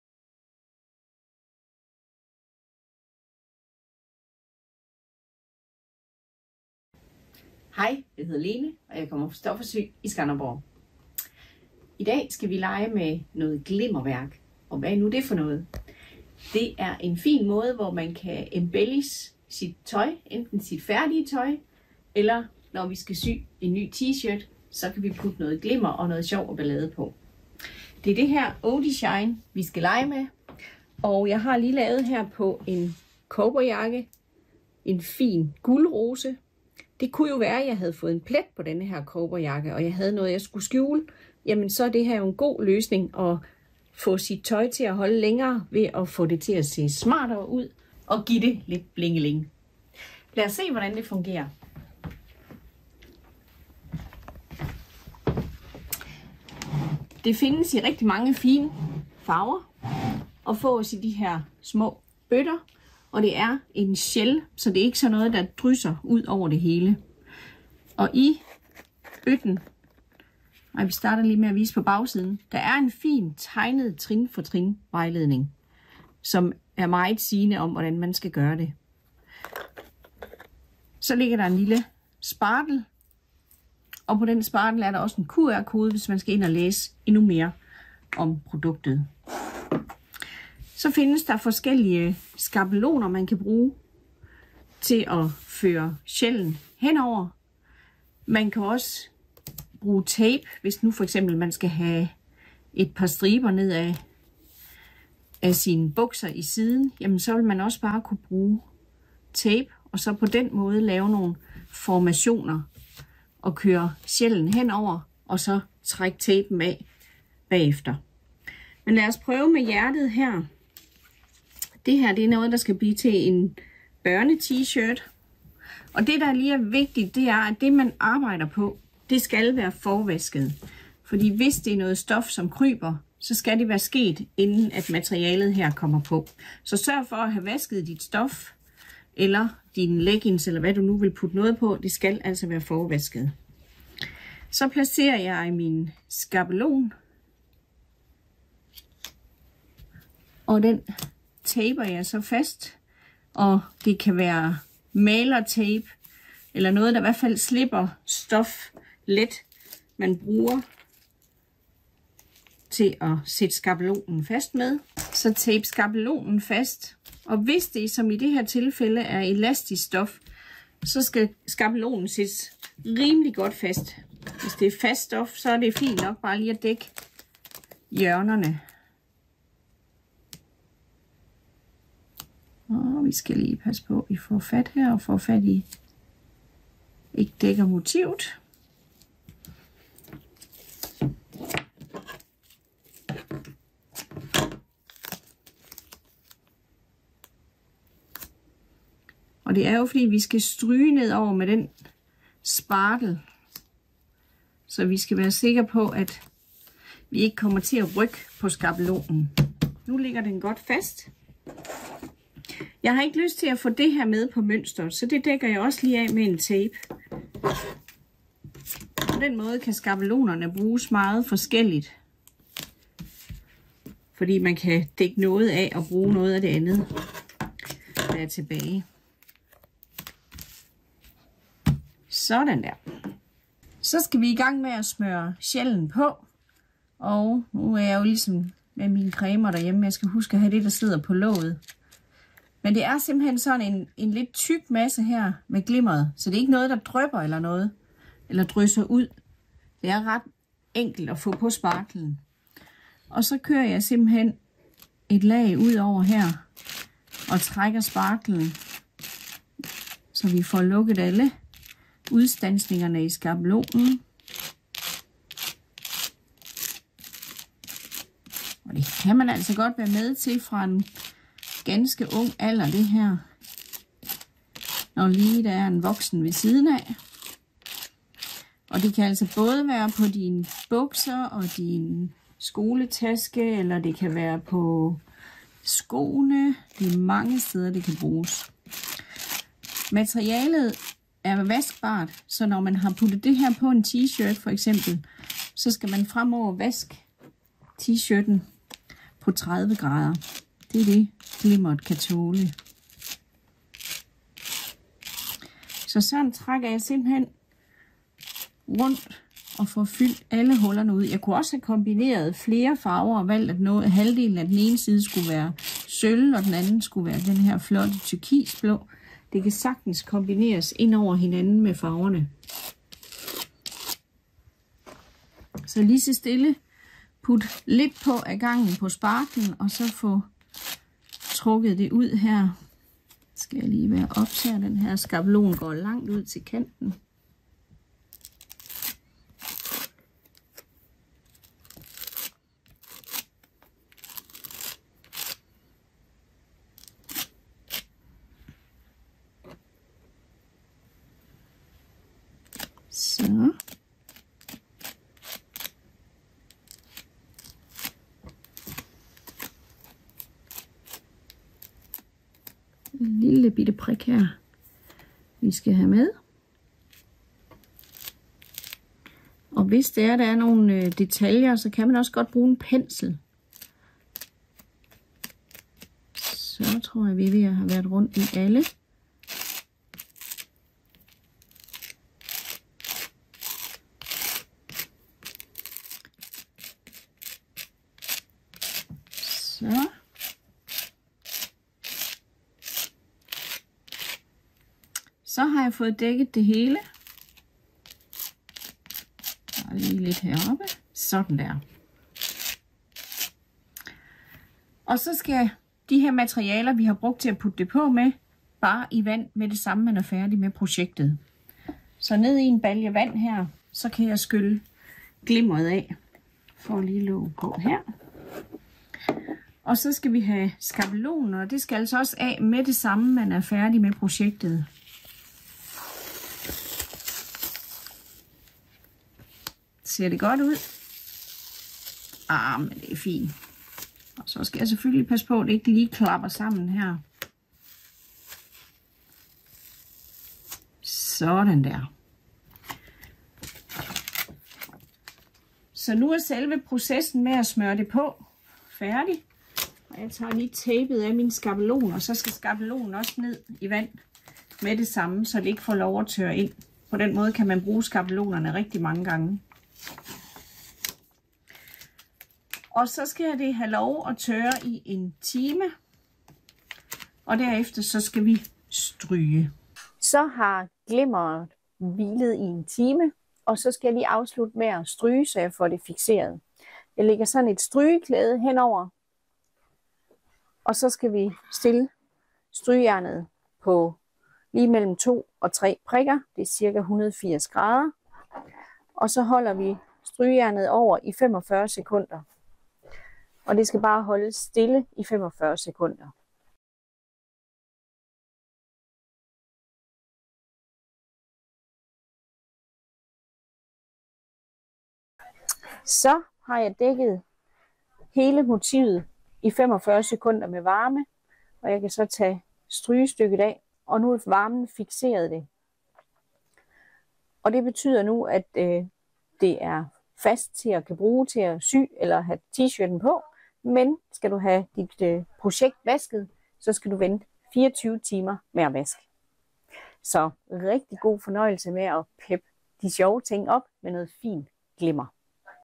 Odishine dekorations glimmer
Odishine dekorations glimmer er en sjov og spændende måde at dekorere tekstil på.